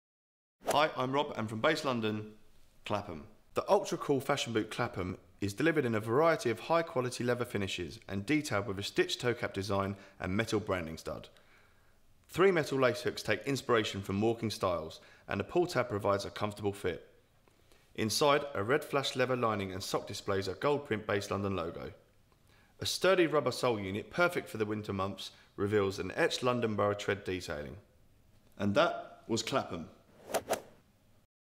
Base London AW15 - Clapham
The ultra cool fashion boot Clapham from Base London is delivered in a variety of high quality leather finishes and detailed with a stich toe cap design and metal branding stud. Three metal lace hooks take inspiration from walking styles and a pull tab provides a comfortable fit.

Inside a red flash leather lining and sock displays a gold print Base London logo.

A sturdy rubber sole unit, perfect for the winter months, reveals an etched London borough tread detailing.